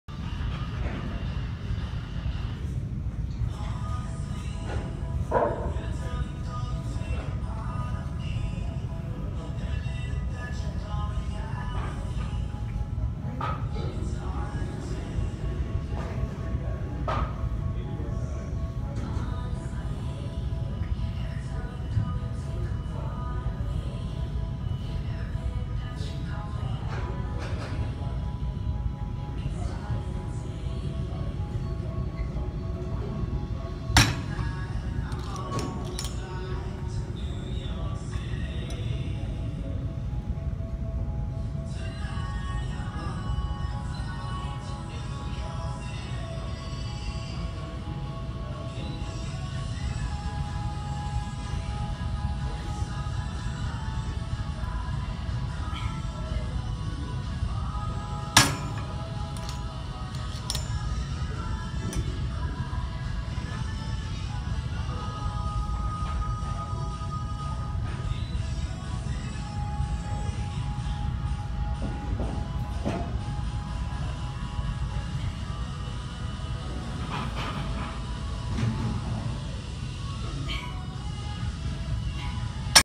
V-Bar Tricep Pushdowns
Pause for a second at the bottom to squeeze the triceps before slowly bringing the bar back up.
Mesh top by Celestial Bodiez
Leggings by DModaSportswear